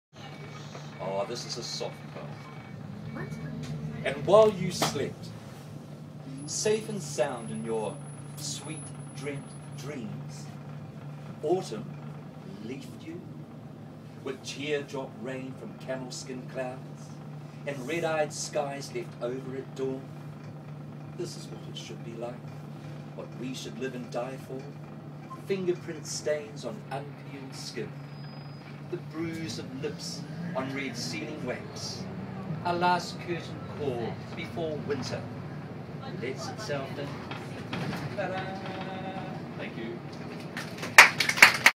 Rowan Reading by the Church (Poets on Buses 2013)
Rowan reads a contemplative piece to the kids.